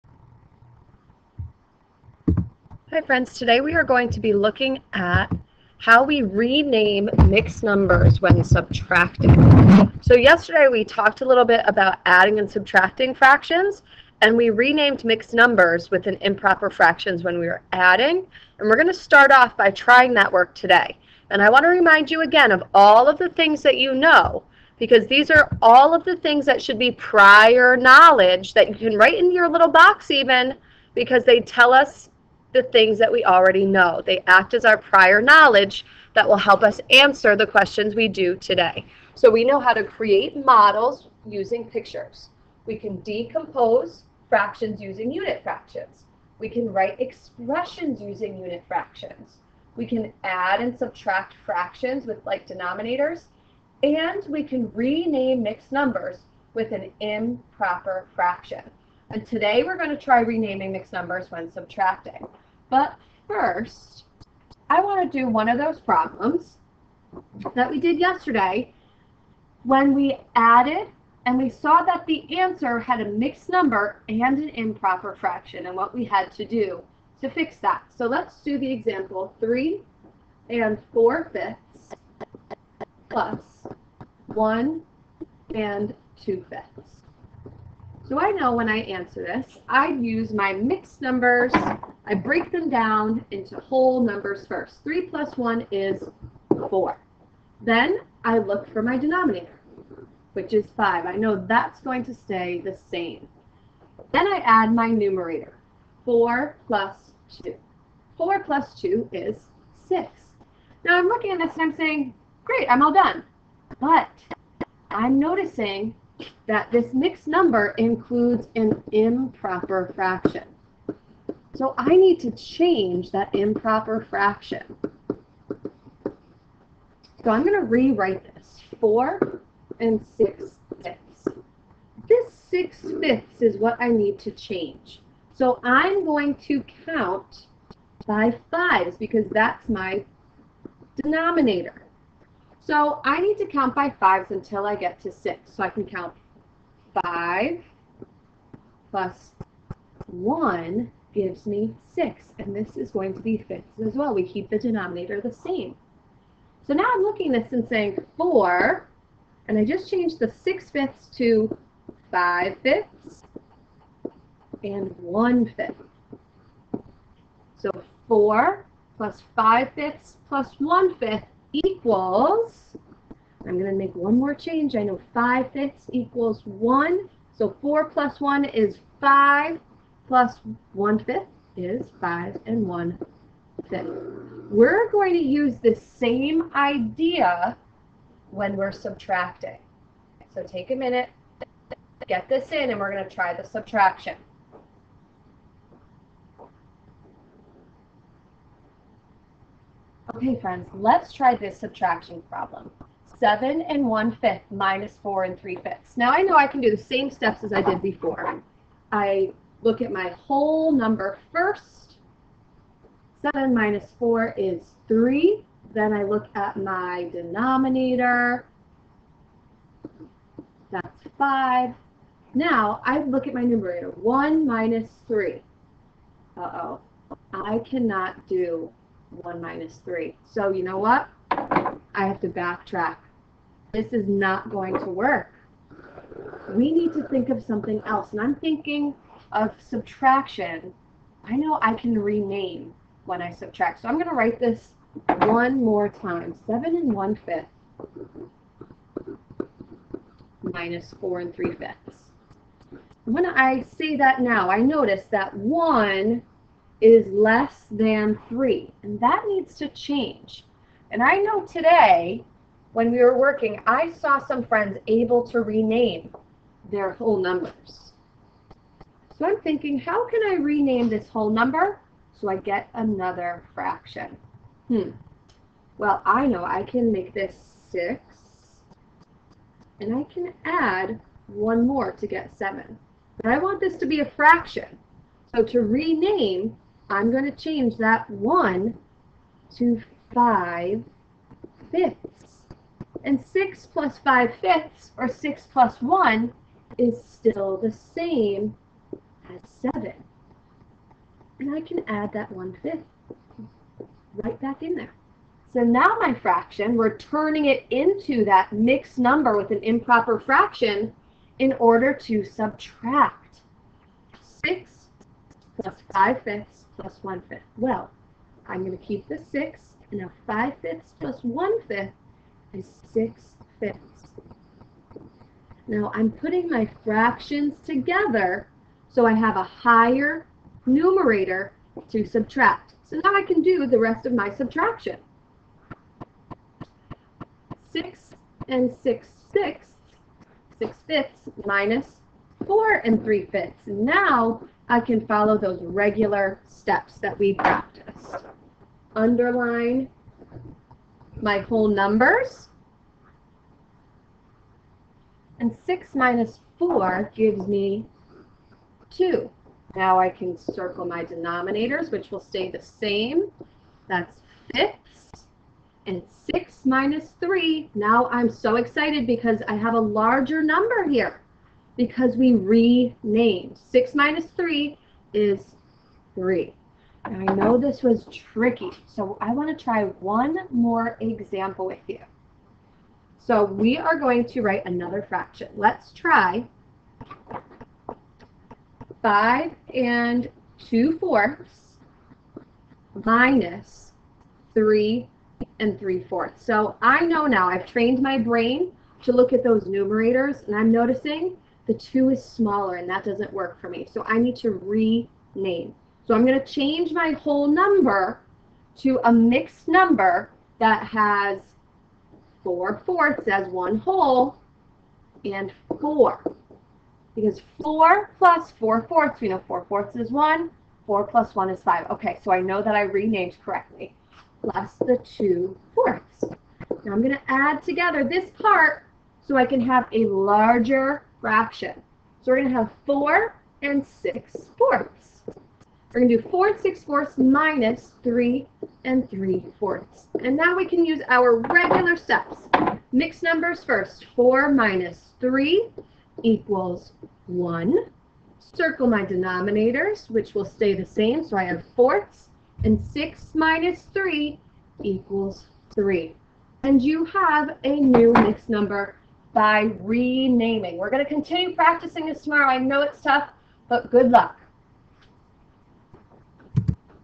Subtract Mixed Numbers with Renaming
We are renaming whole numbers today to help us subtract mixed numbers. This is a tricky one, so take your time and try your best!!